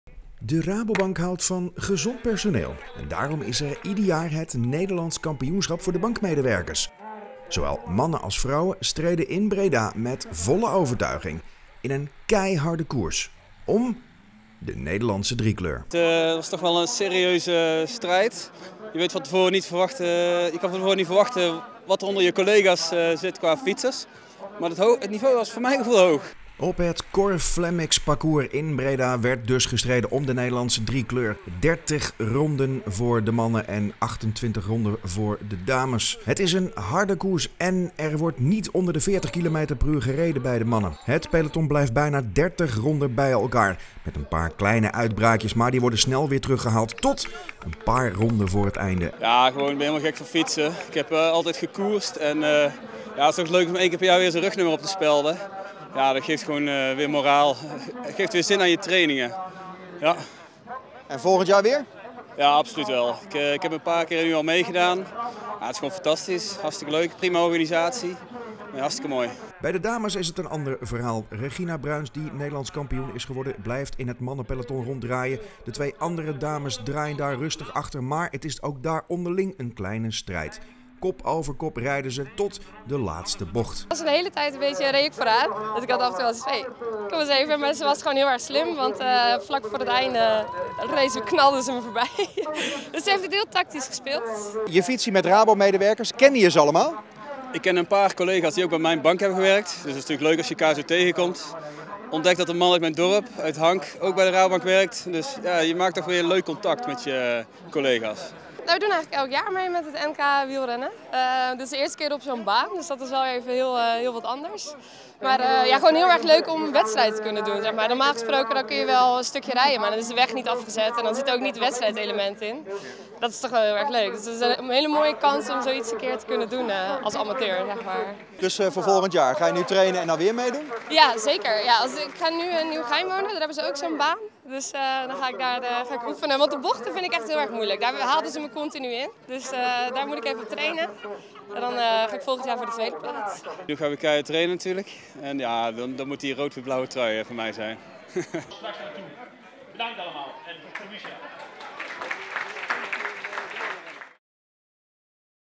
NK Rabo medewerkers
De Rabobank houdt van gezond personeel. Daarom is er ieder jaar het Nederlands Kampioenschap voor de bank medewerkers. Zowel mannen als vrouwen streden in Breda met volle overtuiging in een harde koers om de Nederlandse driekleur